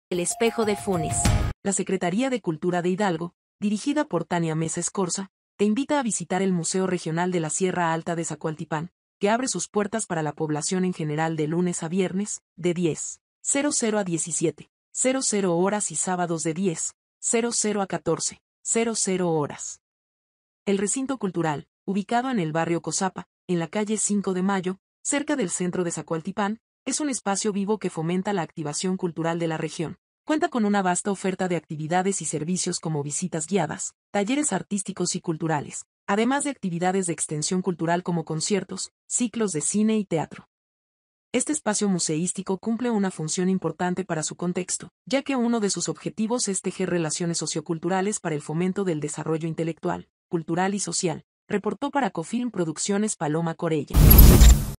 NOTA 7 Museo zacual 05 AGOSTO
NOTA 7
La Secretaría de Cultura de Hidalgo TE INVITA A VISITAR EL Museo Regional de la Sierra Alta de Zacualtipán